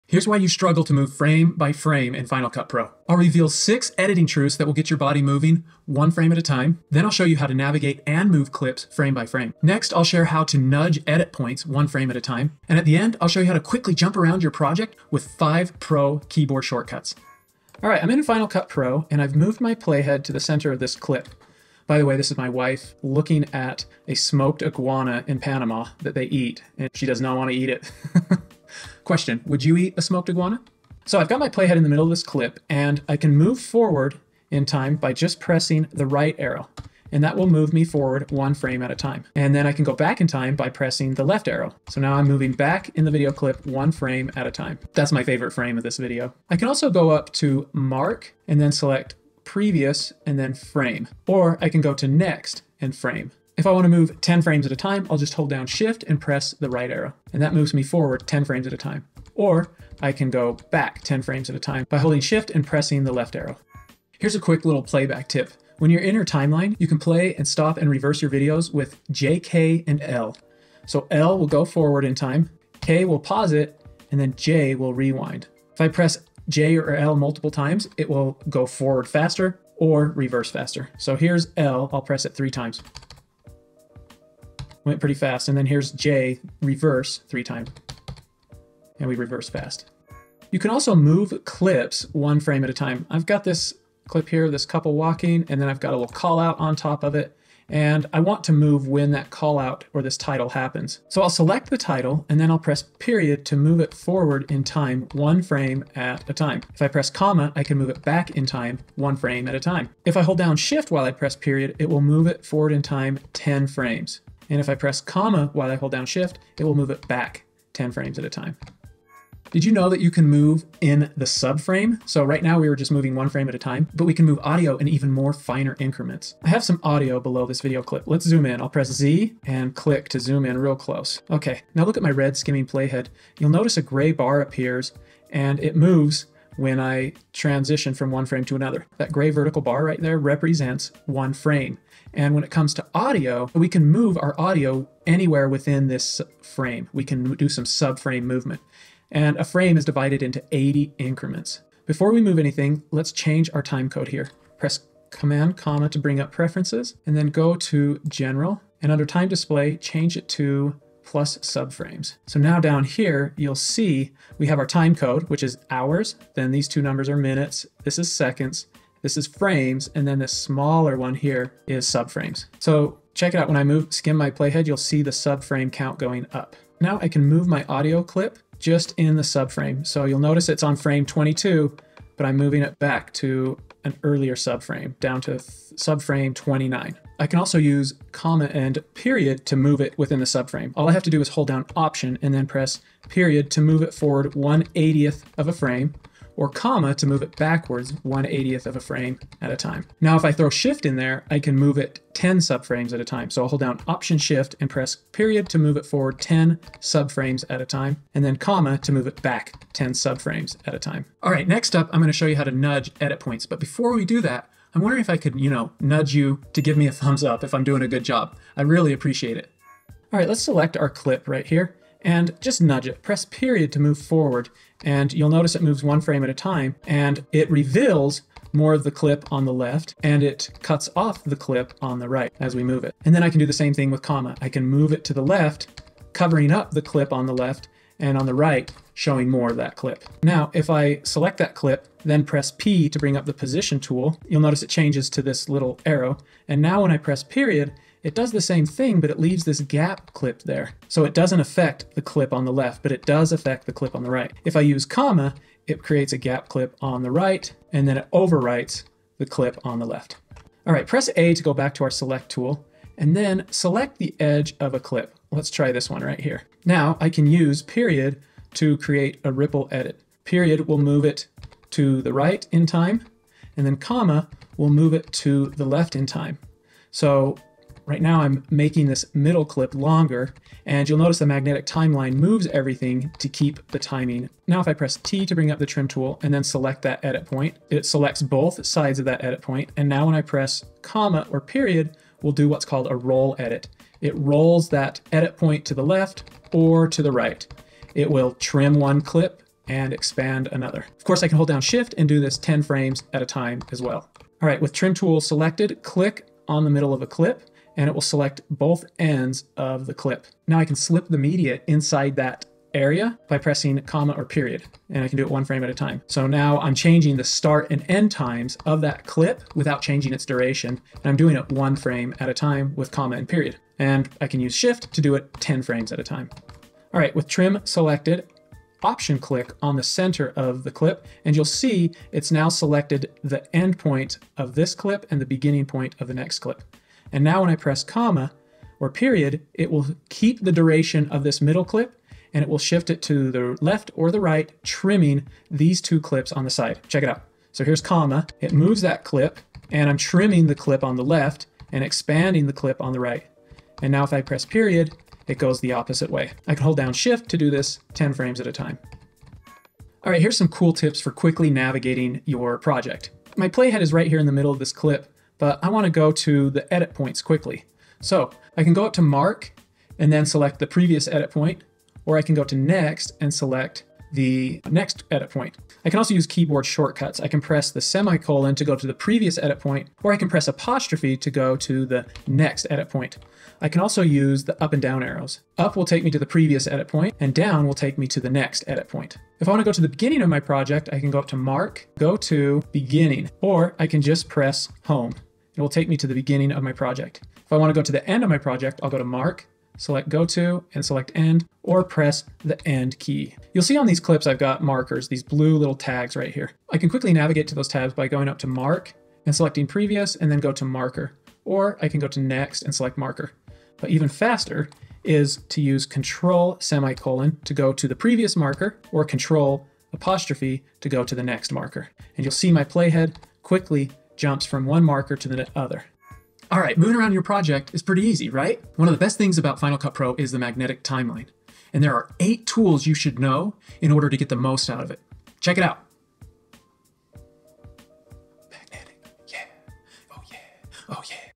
How to Move Frame by Frame in Final Cut Pro
Here’s why you struggle to move frame by frame in Final Cut Pro.  I’ll reveal 6 editing truths that will get your body moving one frame at a time. I’ll show you how to navigate AND move clips frame by frame. Then I’ll share how to nudge edit points one frame at a time and at the end I’ll show you how to quickly jump around your project with 5 pro shortcuts.

00:00 - Intro
00:17 - Right and left arrows
01:03 - Fast playback
01:30 - Moving clips frame by frame
01:59 - Move in the subframe
03:39 - Nudge edit points
05:59 - Navigate by jumping
07:06 - Outro